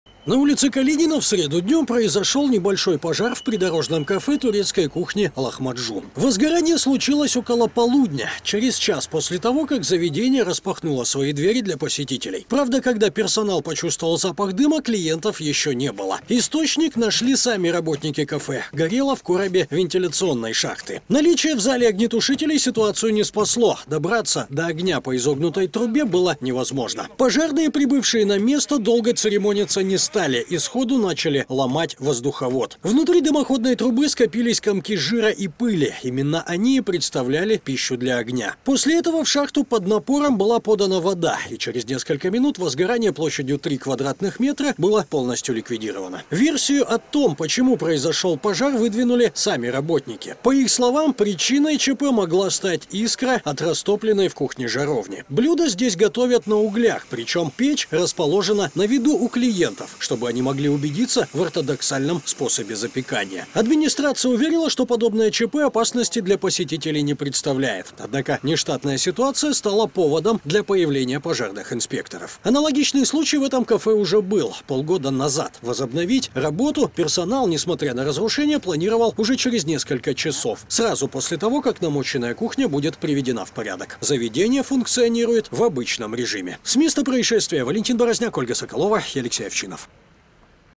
Кафе турецкой кухни загорелось в центре города.MestoproTV
На улице Калинина в среду днем произошел небольшой пожар в придорожном кафе турецкой кухни «Лахмаджун», сообщает портал «Губерния». Возгорание случилось около полудня - через час после того, как заведение распахнуло свои двери для посетителей. Правда, когда персонал почувствовал запах дыма, клиентов еще не было.